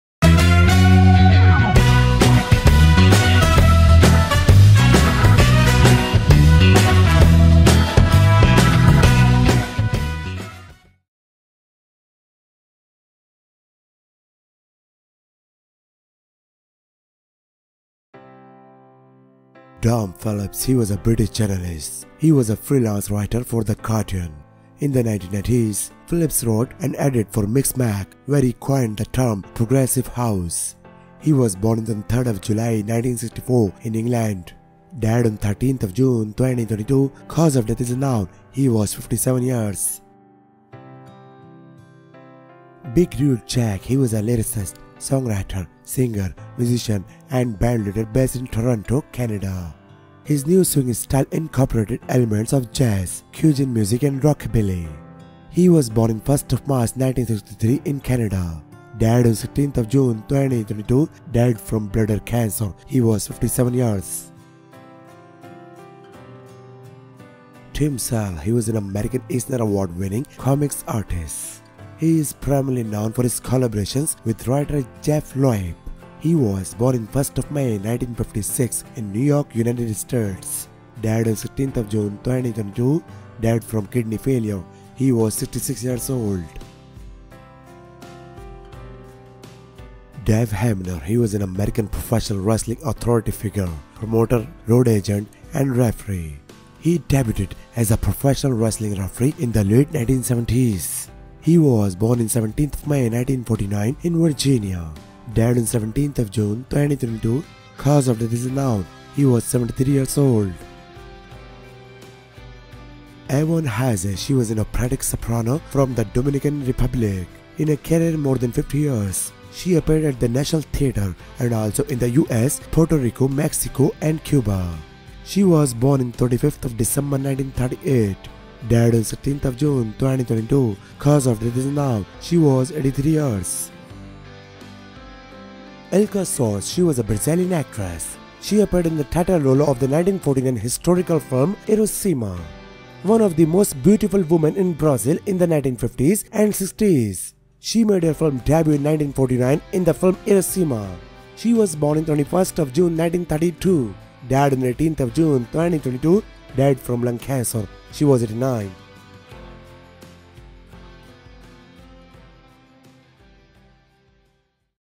Famous People Who Died Today 19 June 2022
Famous people who died today on 19th June 2022.

Ilka Soares
Dave Hebner
Dom Phillips
Big Rude Jake
Tony Boskovic
Tim Sale
Ivonne Haza